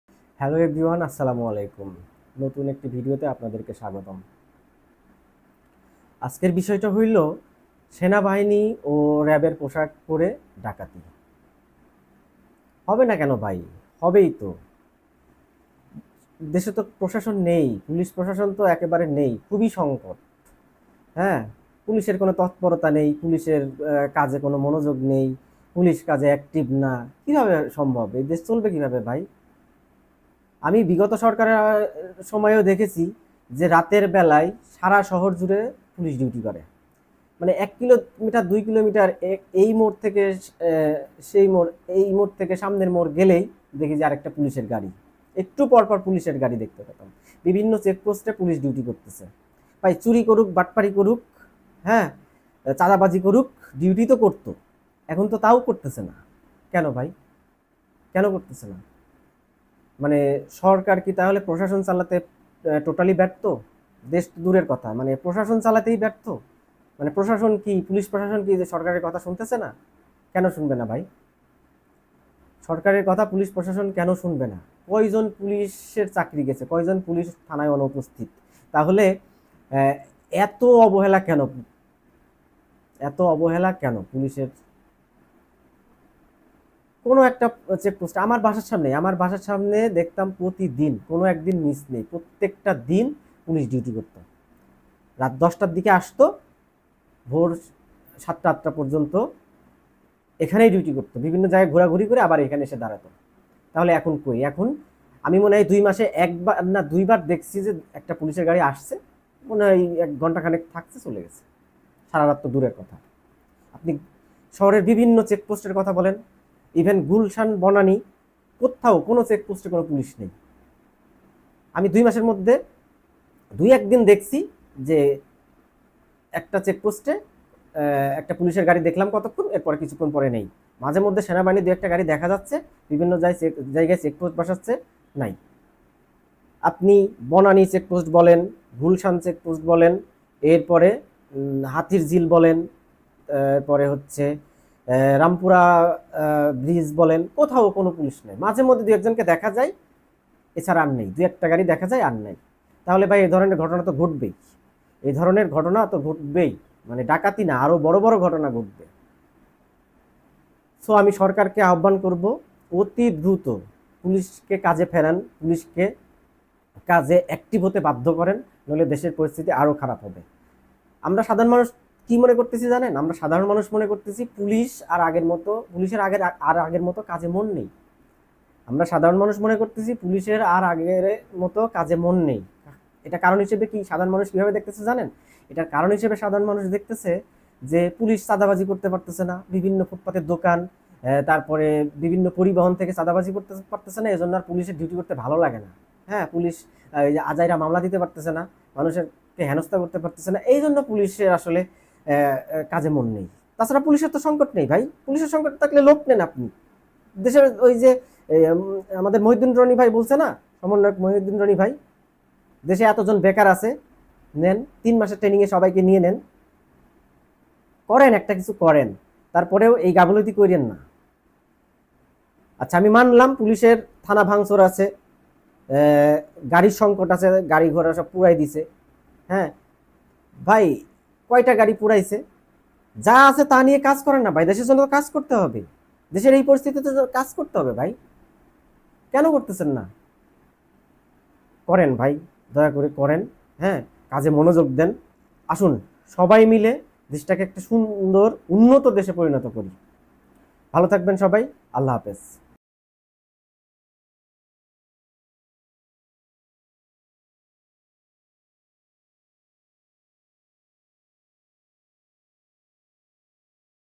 সেনাবাহিনী ও র‍্যাবের পোশাক পরে দূরধর্ষ ডাকাতি
রাজধানীতে সেনাবাহিনী ও র‍্যাবের পোশাক পরে ডাকাতি |